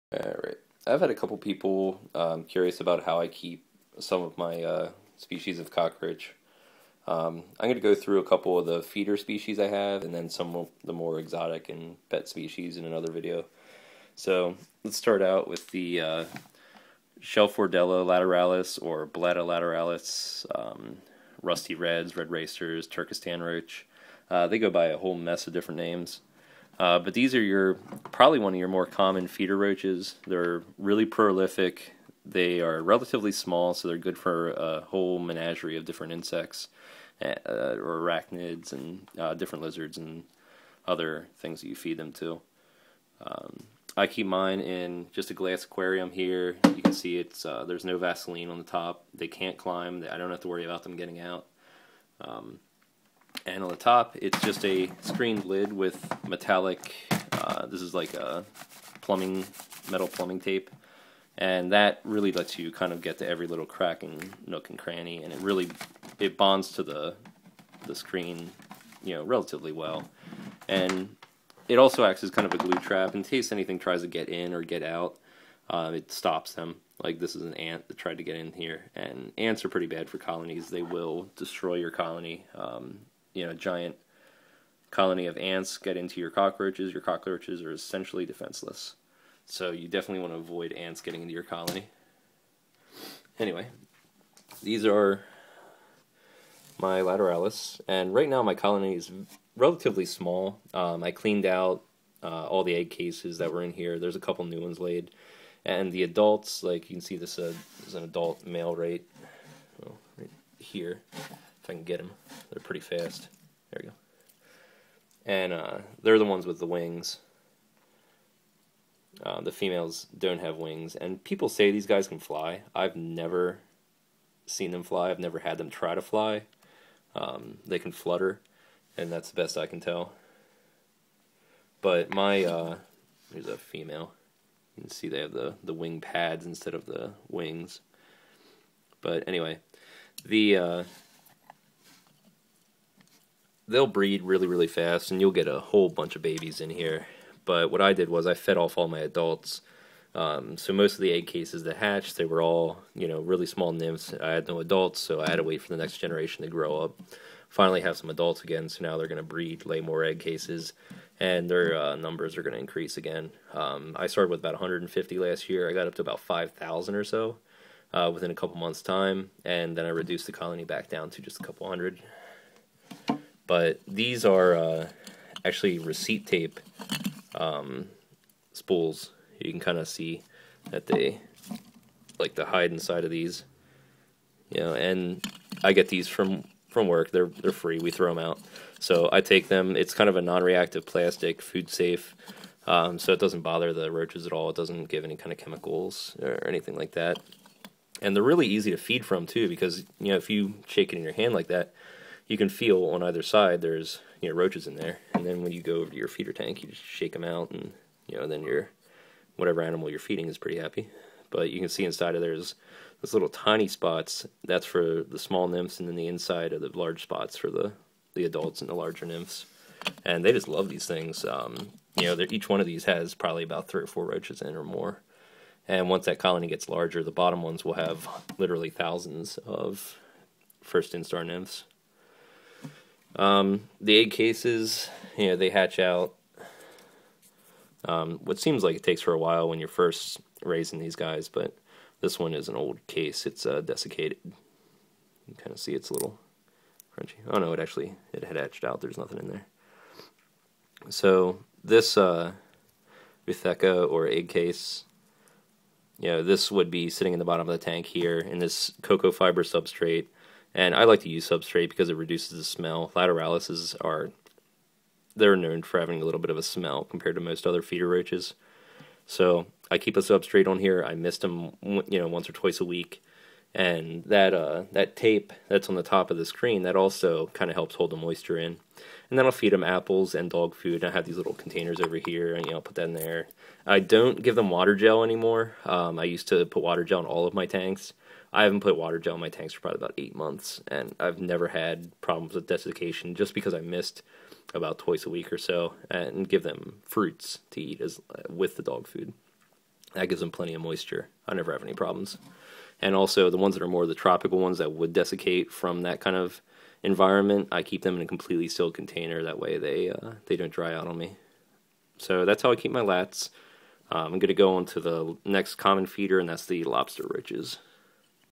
Blatta lateralis cockroach colony care
This and the following videos are some of my feeder and pet cockroach colonies. I go over what I do and why I think it works for them. This is my Blatta (shelfordella) lateralis roach colony.